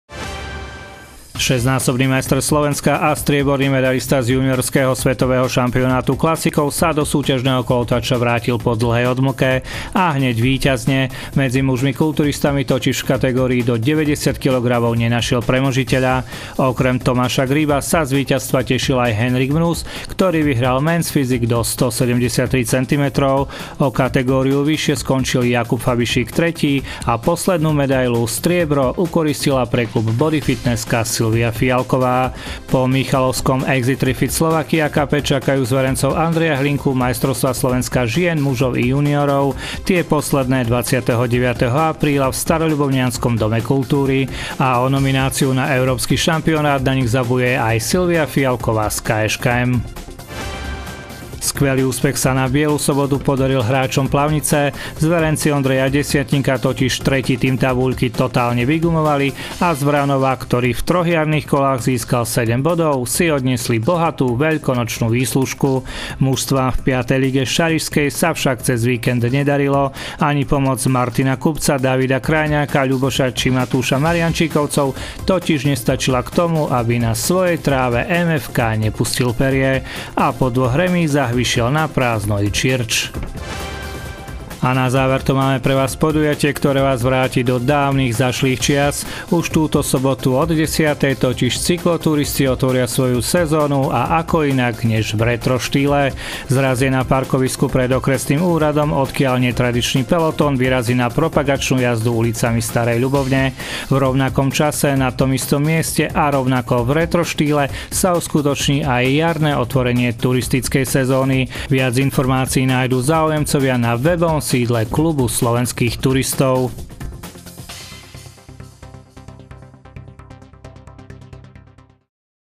Športové správy
Štyri medaily - dvakrát zlato, jedno striebro i bronz získali pretekári Klubu športovej kulturistiky mesta na svojej prvej tohtoročnej súťaži. Aj keď sa ich v Michalovciach predstavilo osem, stredobodom pozornosti bol hlavne Tomáš Grib – a jeho súťažný návrat.